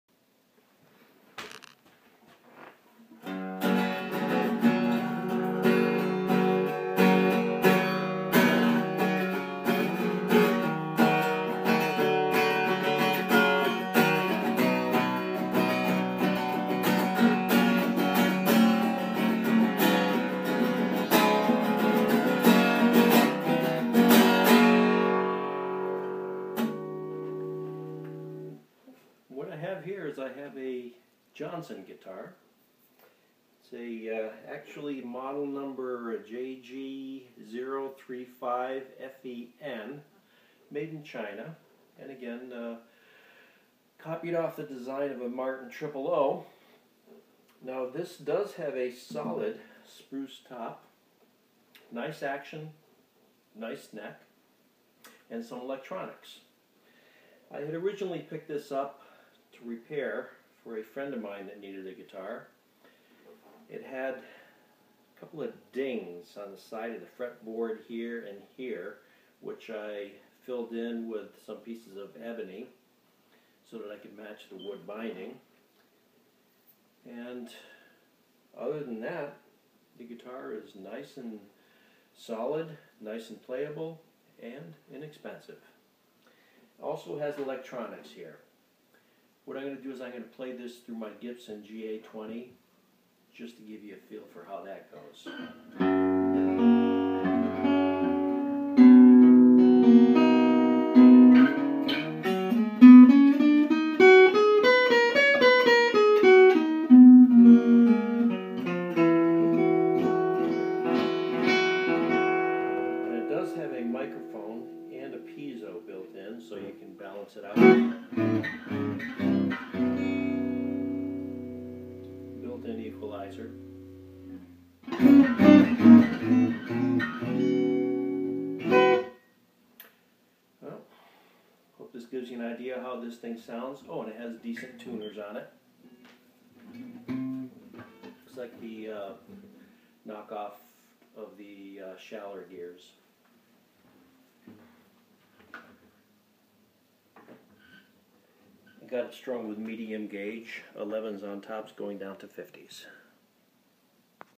Repaired guitar Johnson JG – 035
I picked up this guitar to fix it up for friend, but in the meantime we found another one for her, so this one is up for sale